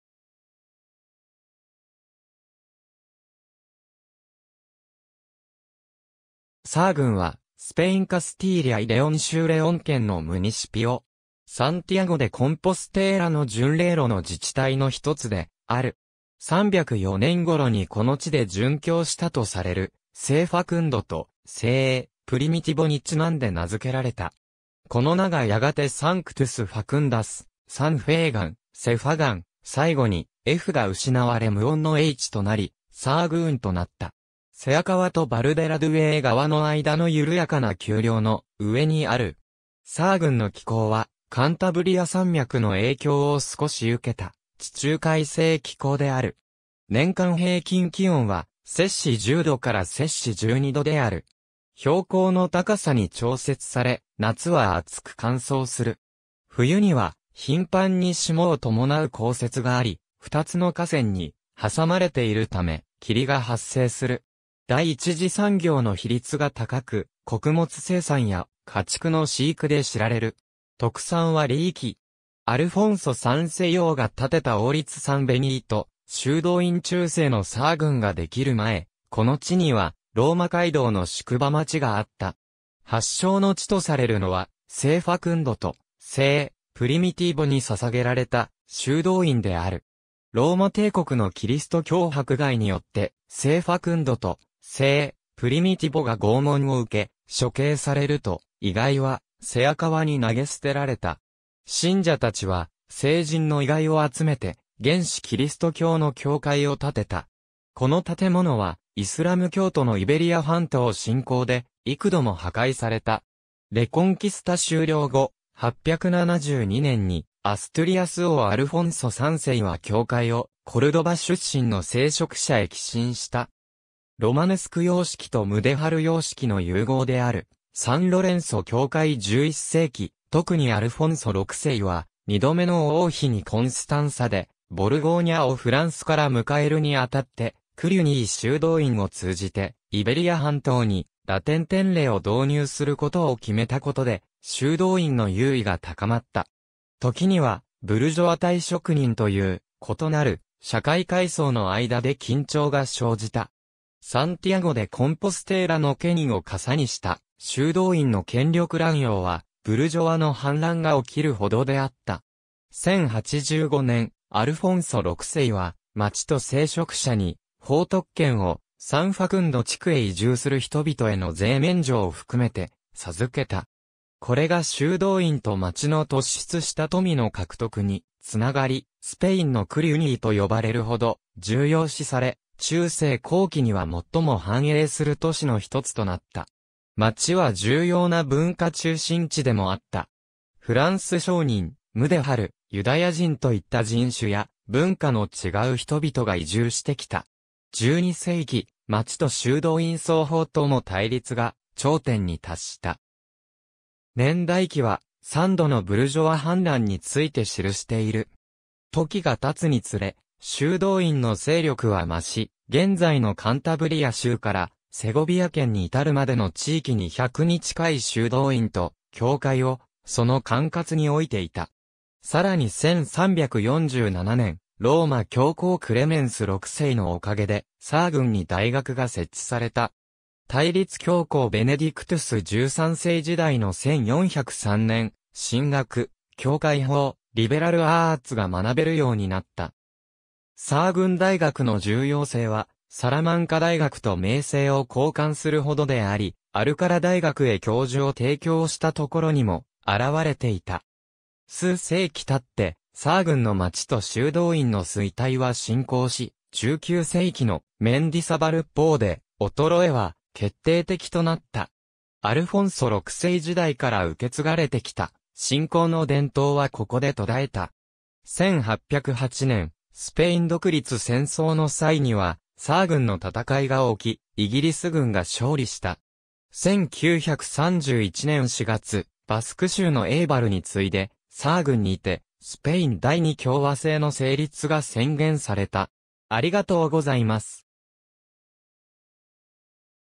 サアグン (スペイン)
カスティーリャ・イ・レオン州の自治体
サアグン（Sahagún [sa(a)ˈɣun]）は、スペイン・カスティーリャ・イ・レオン州レオン県のムニシピオ（基礎自治体）。
サンティアゴ・デ・コンポステーラの巡礼路の自治体の一つである。
304年頃にこの地で殉教したとされる、聖ファクンドと聖プリミティボ（キリスト教徒たちは彼らを Domna Sanctosと呼んでいた）にちなんで名づけられた。
この名がやがてSanctus Facundus、 Sant Fagun、Safagun、最後にfが失われ無音のhとなり、Sahagunとなった。
セア川とバルデラドゥエイ川の間の緩やかな丘陵の上にある。
サアグンの気候は、カンタブリア山脈の影響を少し受けた地中海性気候である。
年間平均気温は10℃から12℃である。
標高の高さに調節され、夏は暑く乾燥する。
冬には頻繁に霜を伴う降雪（年間10日ほど）があり、2つの河川に挟まれているため、霧が発生する。
第一次産業の比率が高く、穀物生産や家畜の飼育で知られる。
特産はリーキ（修道僧が持ち込んだとされる）。
アルフォンソ3世王が建てた王立サン・ベニート修道院 中世のサアグンができる前、この地にはローマ街道の宿場町があった。
発祥の地とされるのは、聖ファクンドと聖プリミティボに捧げられた修道院である。
ローマ帝国のキリスト教迫害によって、聖ファクンドと聖プリミティボが拷問を受け、処刑されると、遺骸はセア川に投げ捨てられた。
信者たちは聖人の遺骸を集めて原始キリスト教の教会を建てた。
この建物はイスラム教徒のイベリア半島侵攻で幾度も破壊された。
レコンキスタ終了後、872年にアストゥリアス王アルフォンソ3世は教会をコルドバ出身の聖職者へ寄進した。
ロマネスク様式とムデハル様式の融合である、サン・ロレンソ教会 11世紀、特にアルフォンソ6世は、二度目の王妃にコンスタンサ・デ・ボルゴーニャをフランスから迎えるにあたって、クリュニー修道院を通じてイベリア半島にラテン典礼を導入することを決めたことで、修道院の優位が高まった。
時には、ブルジョワ対職人という、異なる社会階層の間で緊張が生じた。
サンティアゴ・デ・コンポステーラの権威をかさにした修道院の権力乱用は、ブルジョワの反乱が起きるほどであった。
1085年、アルフォンソ6世は町と聖職者に法特権を、サン・ファクンド地区へ移住する人々への税免除を含めて授けた。
これが修道院と町の突出した富の獲得につながり、『スペインのクリュニー』と呼ばれるほど重要視され、中世後期には最も繁栄する都市のひとつとなった。
町は重要な文化中心地でもあった。
フランス商人、ムデハル、ユダヤ人といった人種や文化の違う人々が移住してきた。
12世紀、町と修道院双方とも対立が頂点に達した。
年代記は、3度のブルジョワ反乱について記している。
時がたつにつれ、修道院の勢力は増し、現在のカンタブリア州からセゴビア県に至るまでの地域に100に近い修道院と教会をその管轄においていた。
さらに1347年、ローマ教皇クレメンス6世のおかげで、サアグンに大学が設置された。
対立教皇ベネディクトゥス13世時代の1403年、神学、教会法、リベラル・アーツが学べるようになった。
サアグン大学の重要性は、サラマンカ大学と名声を交換するほどであり、アルカラ大学へ教授を提供したところにも現われていた。
数世紀たって、サアグンの町と修道院の衰退は進行し、19世紀のメンディサバル法（教会、修道会の大土地所有廃止）で衰えは決定的となった。
アルフォンソ6世時代から受け継がれてきた信仰の伝統はここで途絶えた。
1808年、スペイン独立戦争の際には、サアグンの戦いが起き、イギリス軍が勝利した。
1931年4月、バスク州のエイバルに次いで、サアグンにてスペイン第二共和政の成立が宣言された。